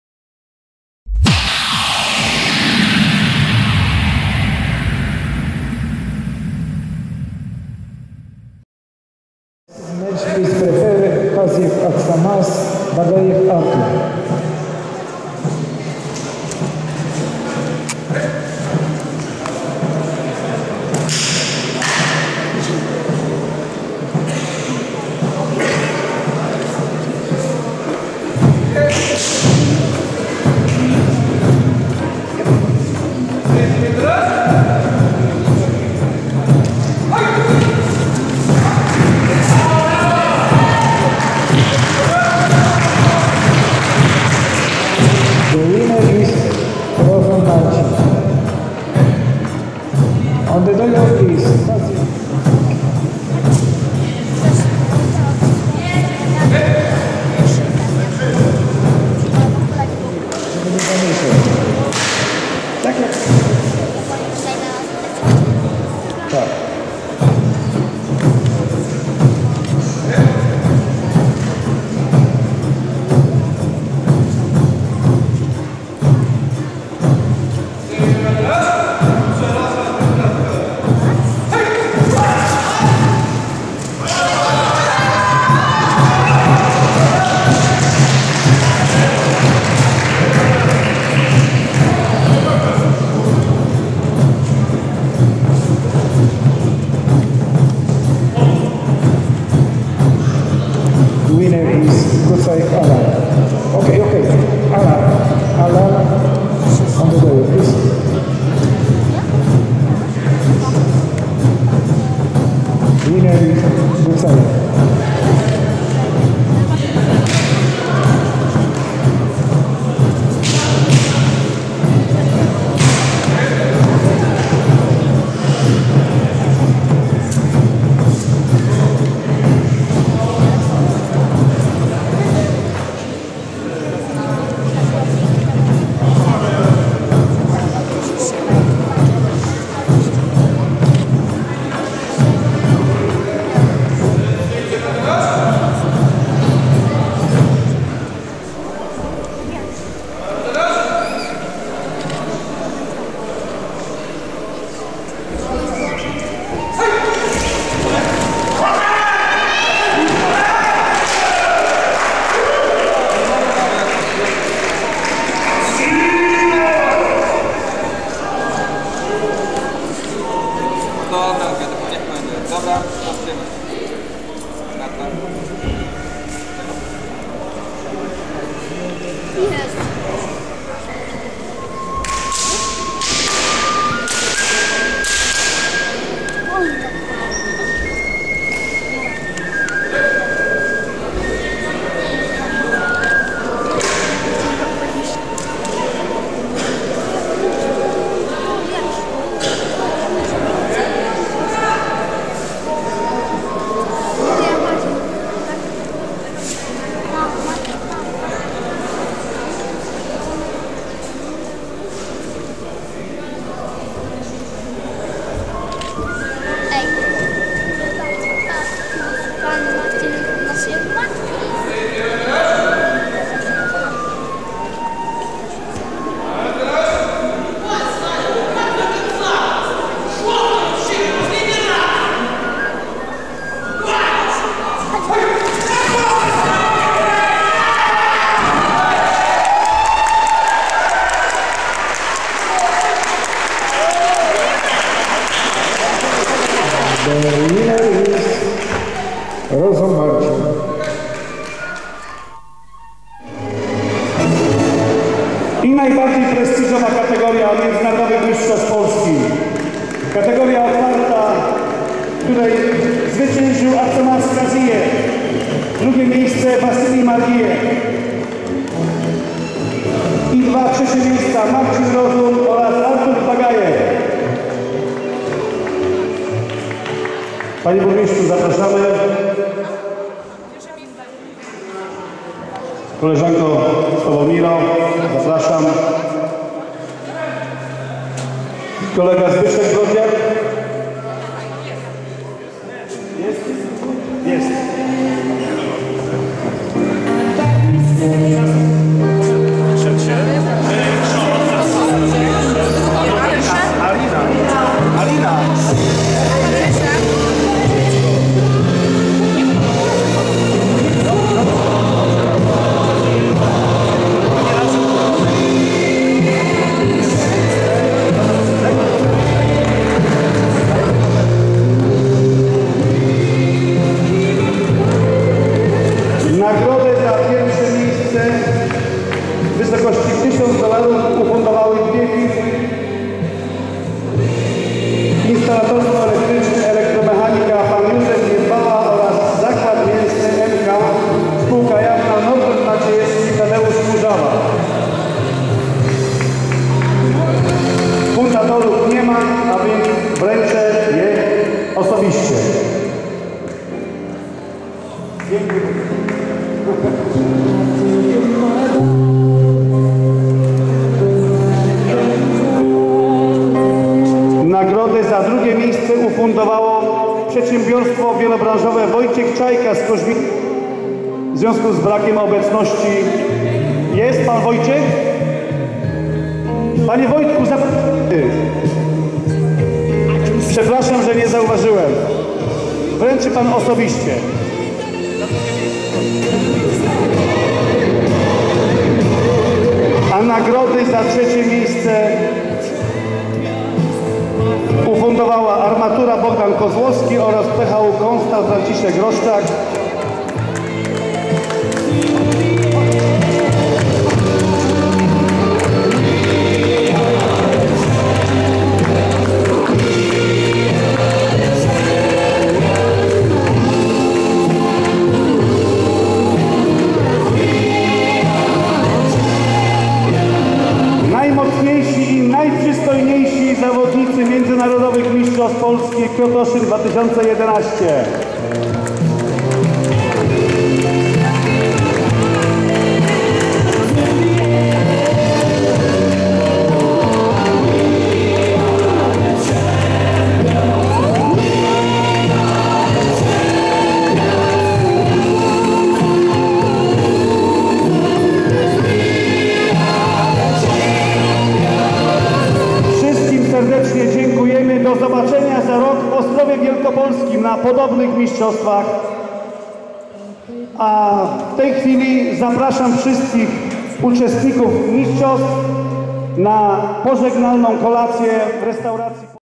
POLAND OPEN SUMO 2011 - Man OPEN FINAL
Poland Open w Sumo. Serdecznie zapraszamy do obejrzenia videorelacji z Międzynarodowych Mistrzostw Polski w Krotoszynie, które odbyły się 8 października 2011 roku.